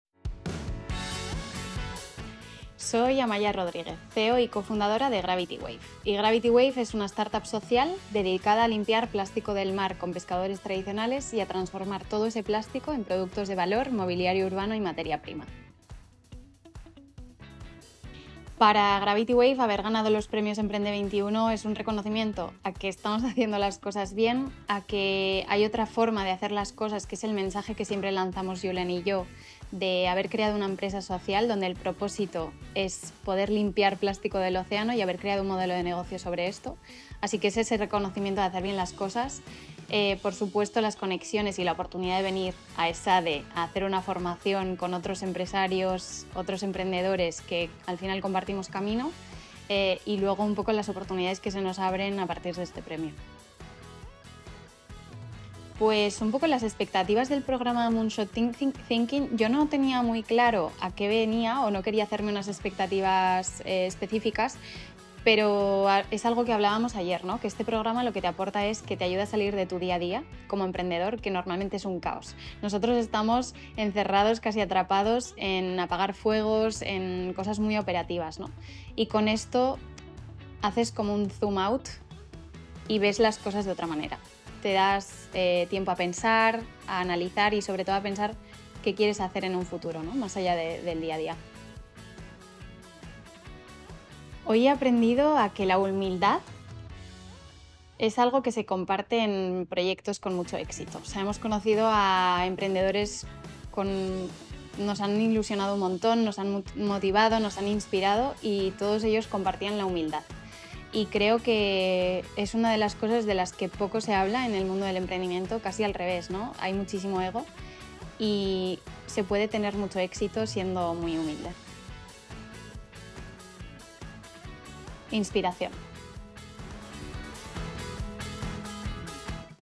Entrevista a Amaia Rodríguez, CEO de Gravity Wave | 17ª edición Premios EmprendeXXI
Entrevista a Amaia Rodríguez, CEO de Gravity Wave, startup ganadora de la Comunidad Valenciana de la 17ª edición de Premios EmprendeXXI.